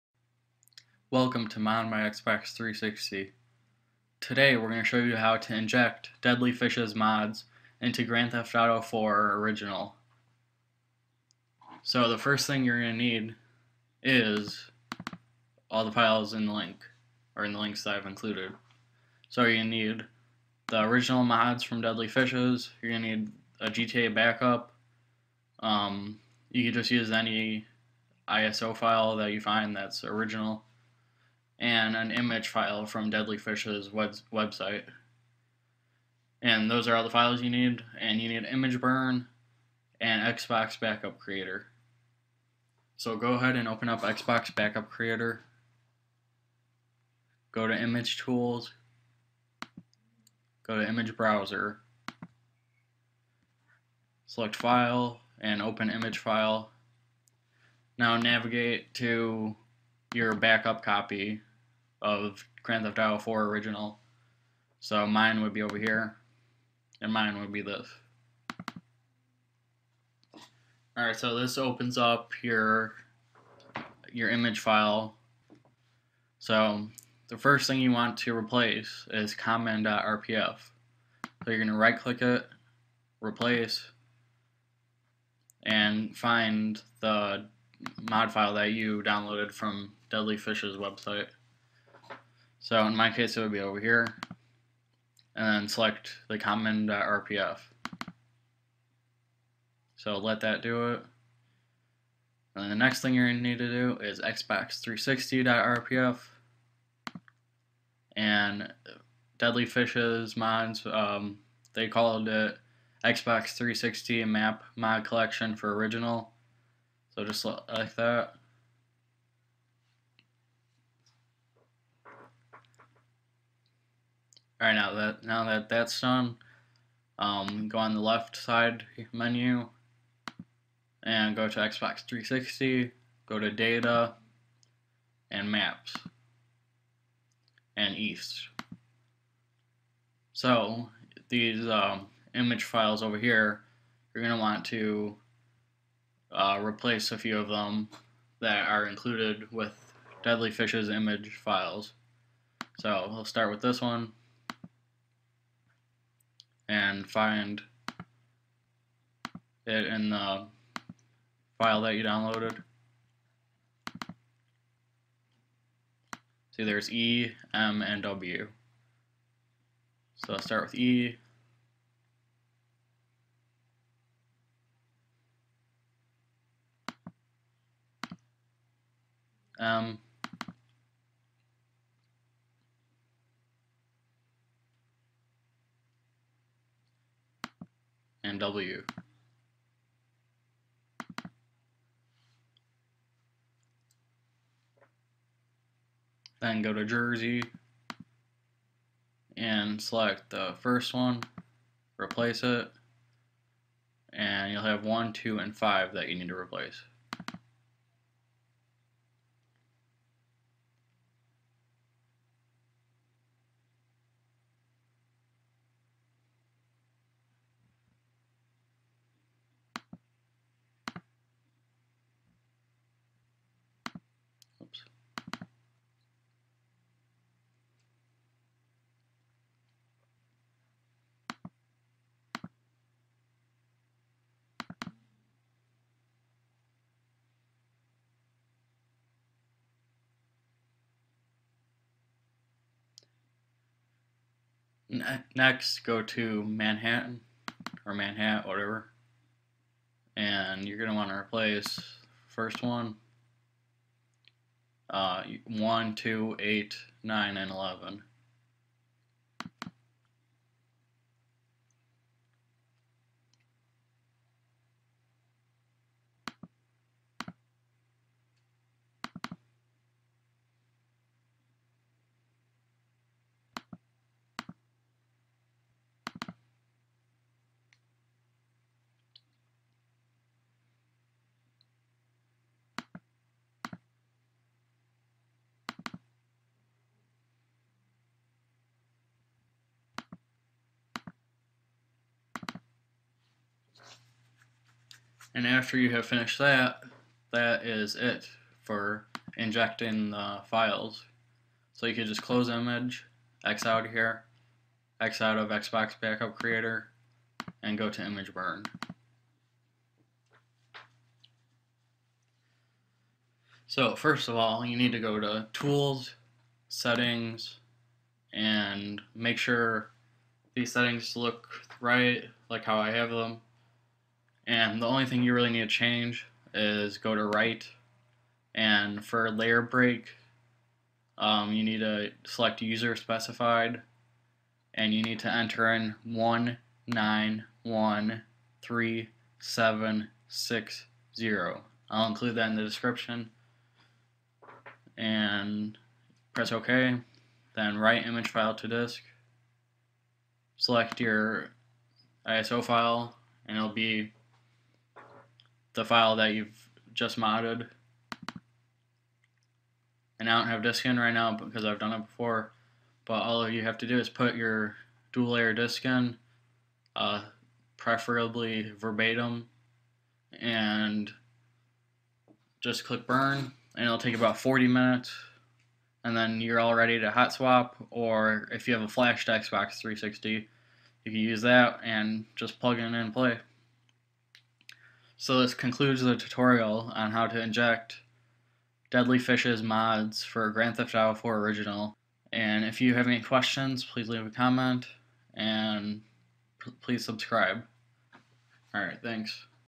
GTA IV ISO Modding Tutorial Injecting Deadly Fishes Mod Files !
If you have questions please comment or send a message. Thanks!

These Mods Are After Patch & Are Confirmed Working As Of 3/11/12

You will need:

User Specified Sector Code For ImgBurn: 1913760